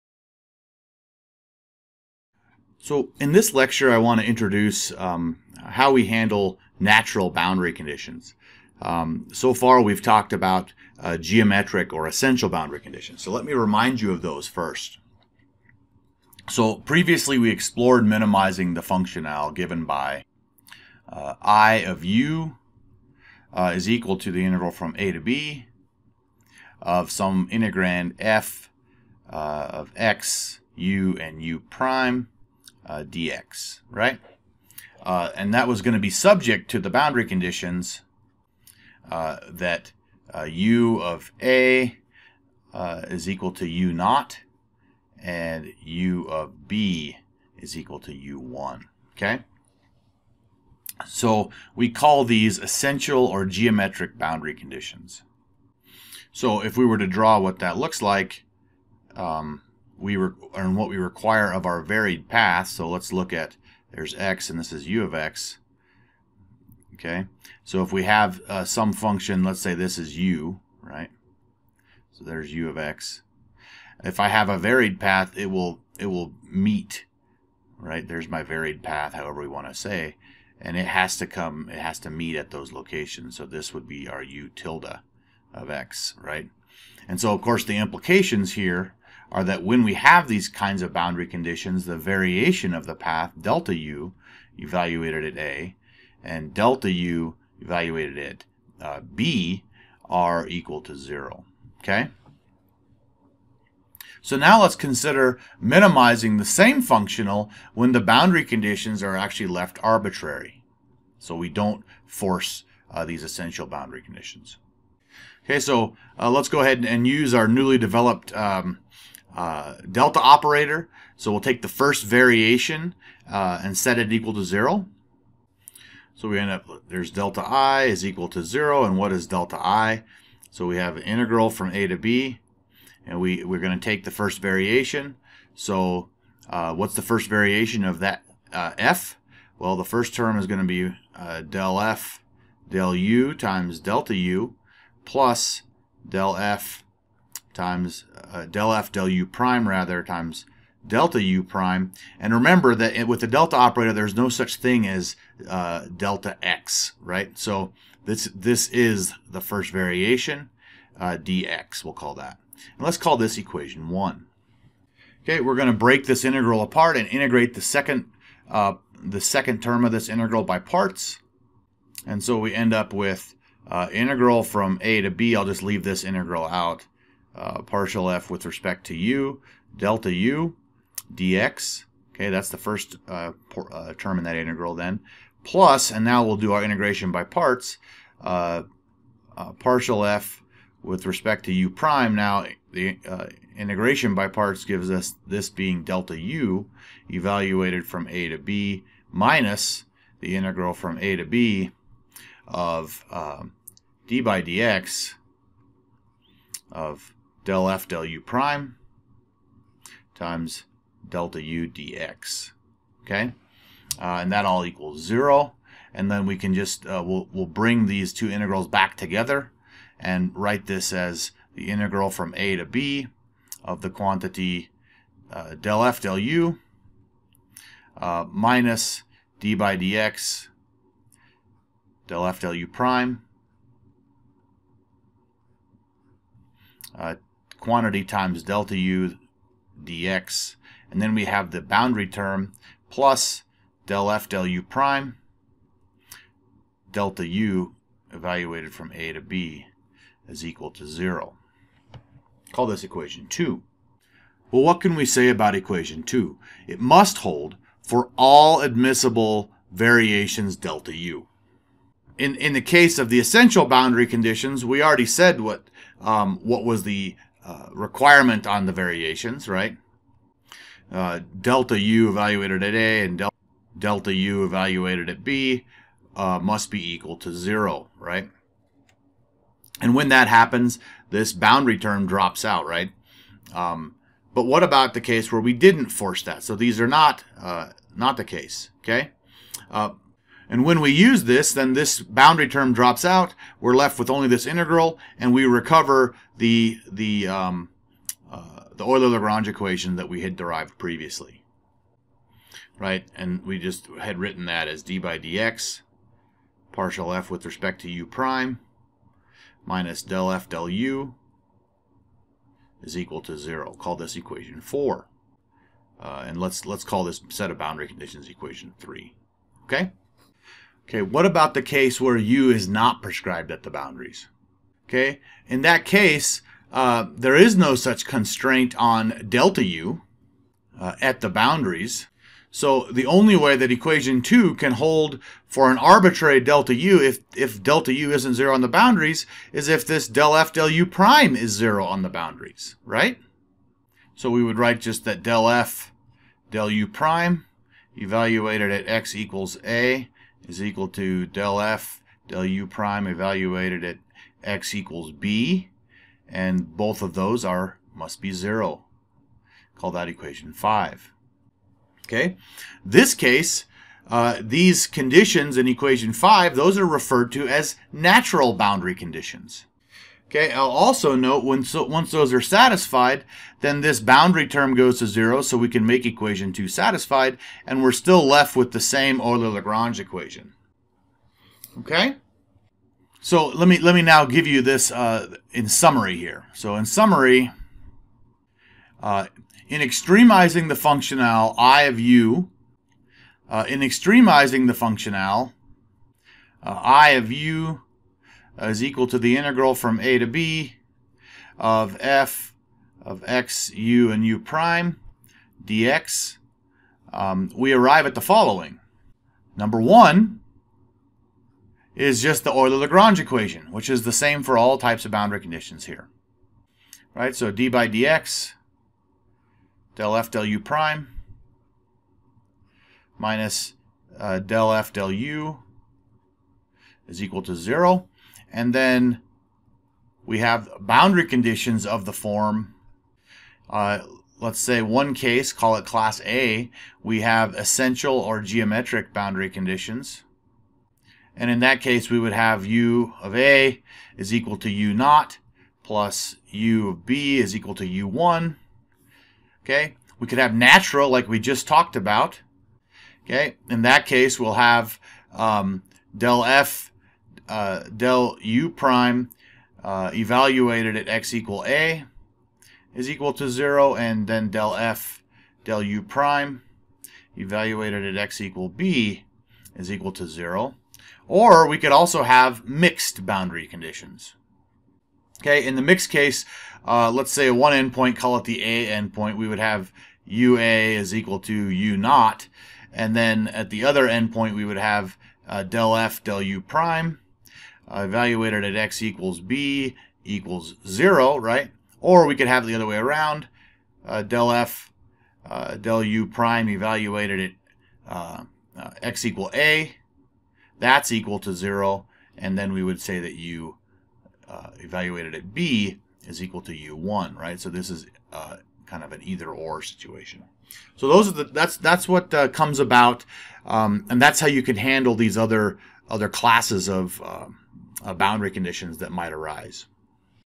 Variational Methods (Natural Boundary Conditions)
Considers the case where the function is not prescribed on the boundaries so that the variation need not vanish on the boundaries. Develops the necessary boundary conditions, natural BCs, that arise from taking the first variation of the functional.